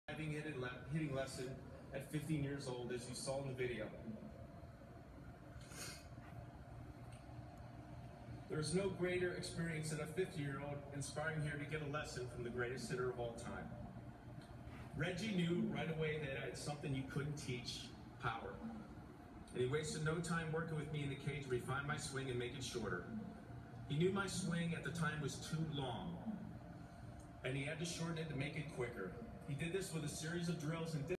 Baseball hall of fame
Baseball Hall of Fame induction ceremony July 24, 2016
This was happening as Mike Piazza was giving his induction speech. The camera was focused on Reggie Smith, you can clearly see behind him, the man in a blue shirt, picking his nose and eating his booger on live tv!